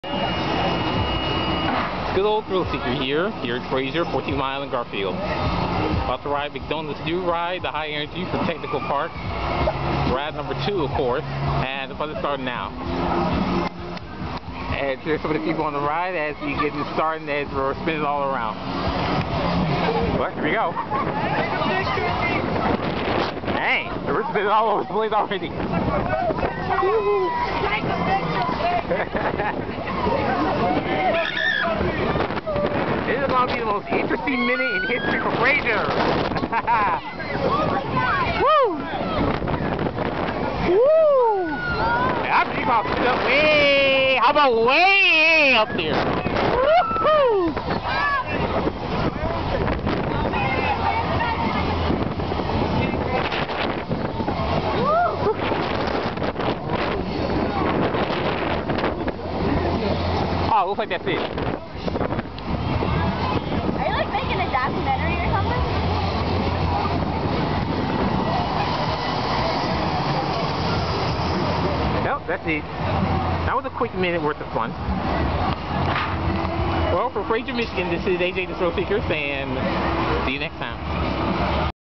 The High Energy - On Ride! Ride 2 at Fraser
This is perhaps the first on ride video on McDonagh's latest ride, and just riding it was something else indeed. When I took a test ride the first time, I was shocked. The second time around was no different, especially heading to its peak.